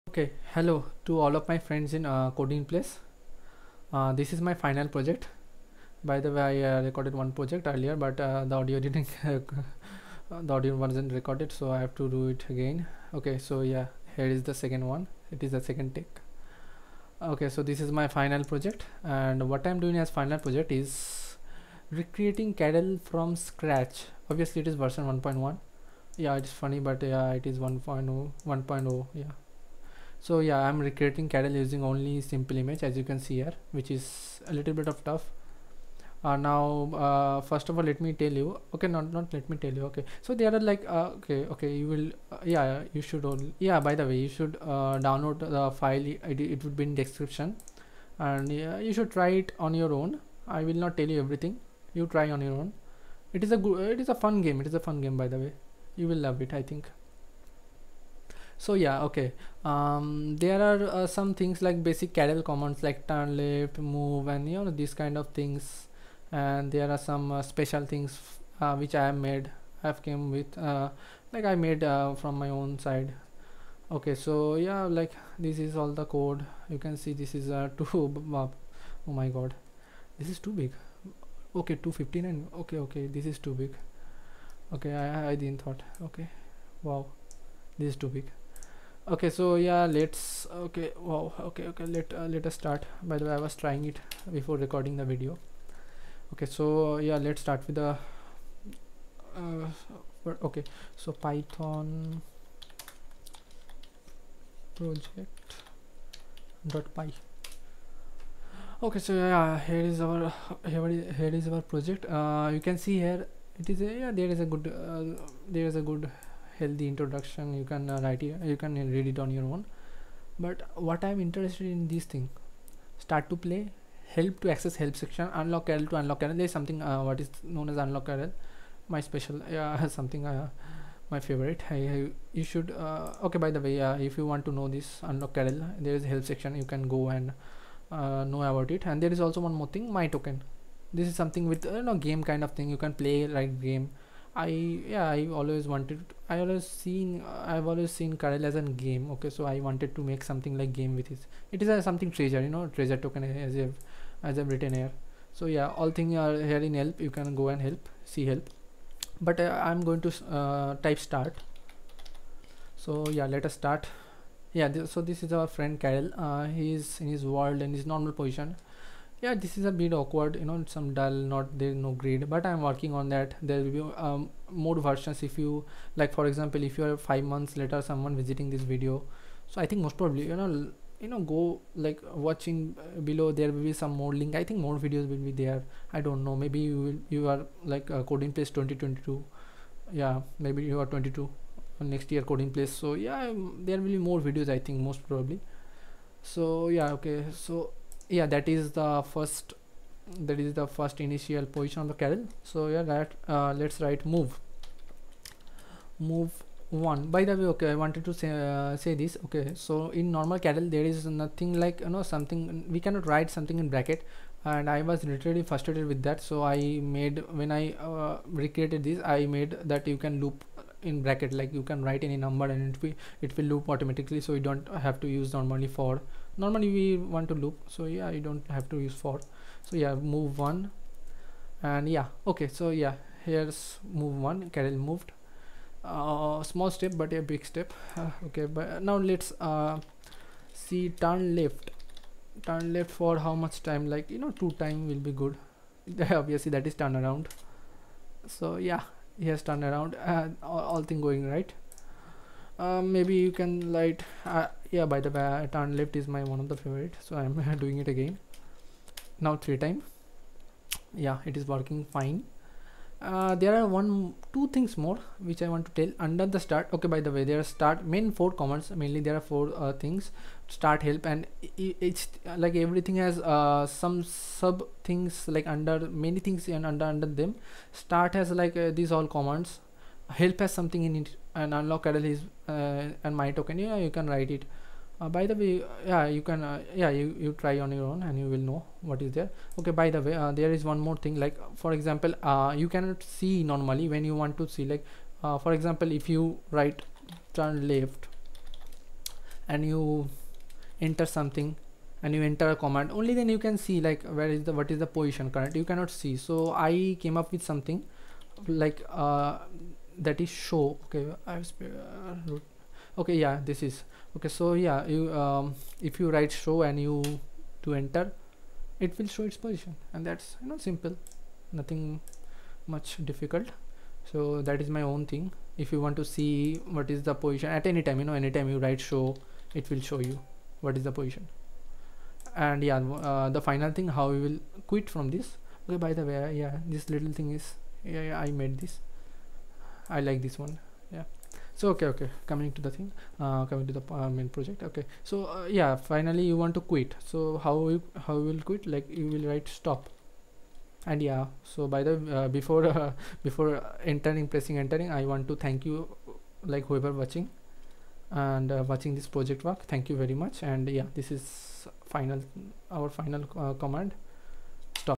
Stanford University Code In Place 2021 Final Project - Recreating Karel The Robot from Scratch
Hi, my name is T.S, and this is my Code In Place final project. By the way, if u don't know, Code In Place is a initiative by Computer Science Department of Stanford University (Chris, Mehran and Julie), where they teach Python to  thousands of students online every year!

In this project, I've recreated Karel from scratch using Simpleimage, conditional loops, lists and strings!

Hope u will like it! And finally, pls don't forget KAREL!!!

python file - haven't uploaded to Google drive yet!! yea, I'm lazy!!😂

contact - search T.S in Ed.
(edit: by hobby, I'm a bedroom producer! And this is a music🎶 channel)